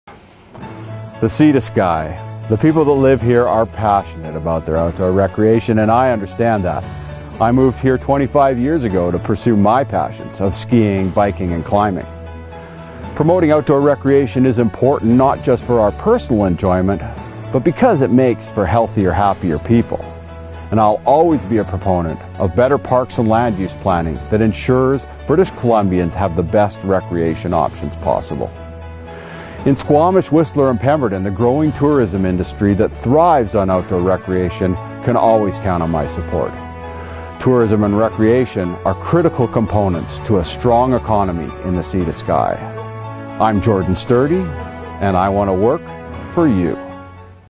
Jordan Sturdy on Outdoor Recreation and the Sea to Sky
Jordan Sturdy is committed to being a strong voice for the Environment and Outdoor recreation in West Vancouver-Sea to Sky.

Authorized by Luke Geiger, Financial Agent for the BC Liberal Party.